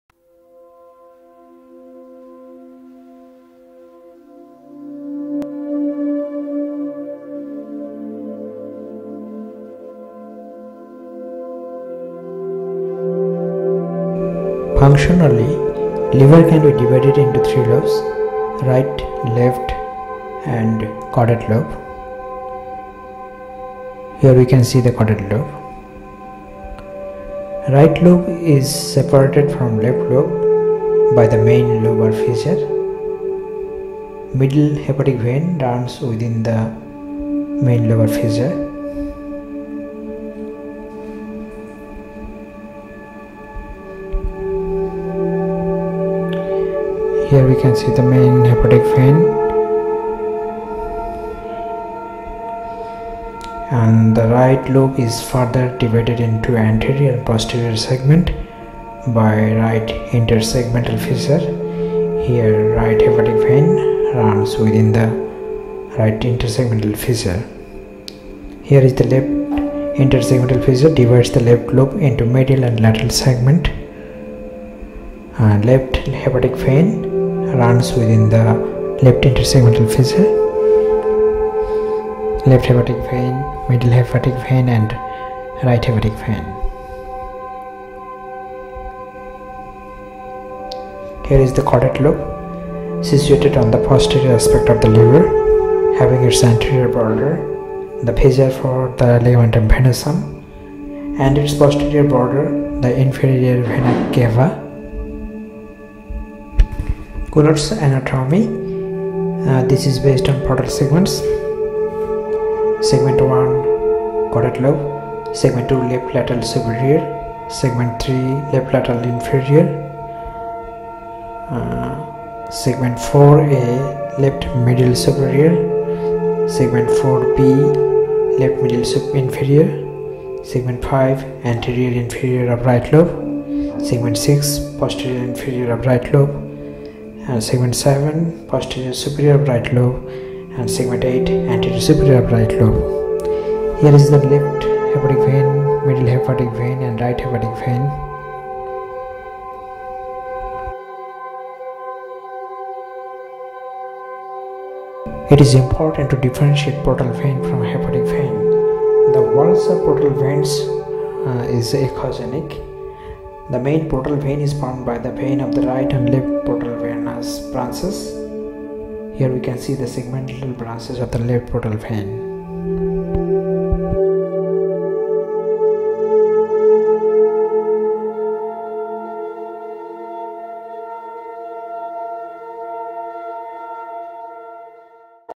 Ultrasonography Tutorial-Sonographic Anatomy of The Liver
Discussing about basic sonographic anatomy of liver,Segments of liver,Hepatic veins,portal veins.